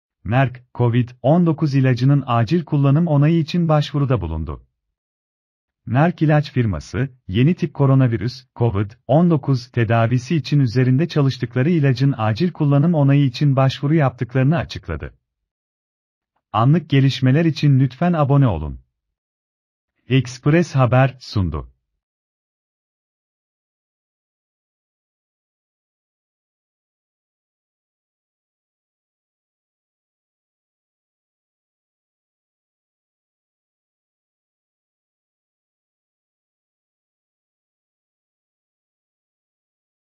Son dakika... Merck, COVID-19 ilacının acil kullanım onayı için başvuruda bulundu
MERCK ilaç firması, yeni tip koronavirüs (COVID-19) tedavisi için üzerinde çalıştıkları ilacın acil kullanım onayı için başvuru yaptıklarını açıkladı.